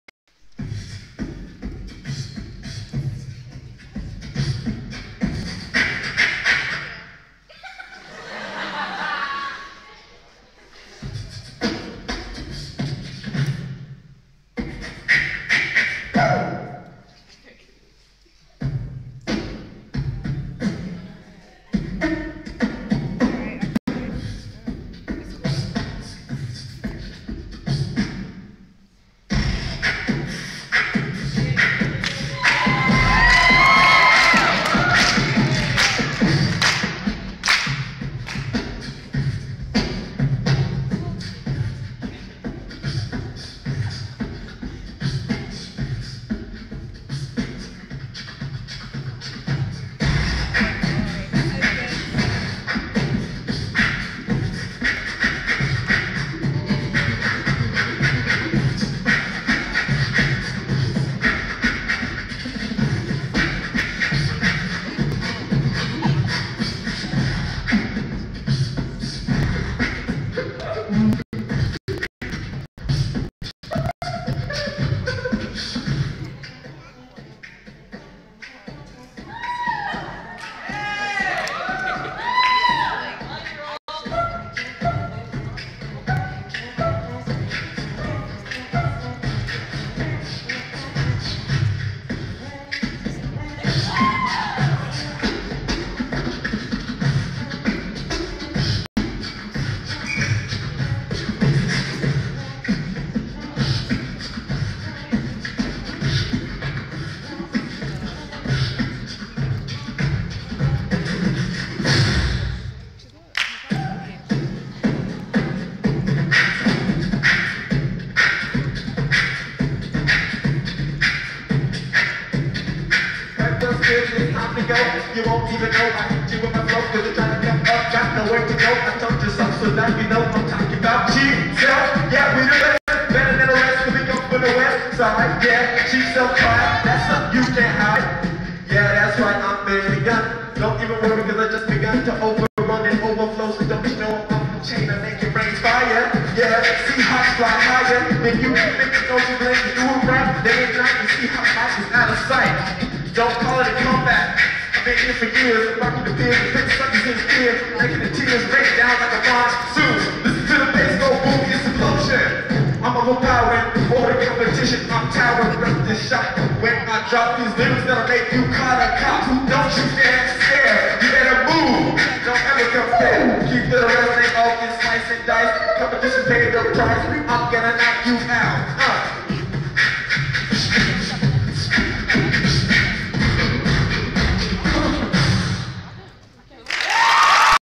April 11.2013 Sealth Talent Night 010
Awesome Rapping by Nehemiah Parker & ?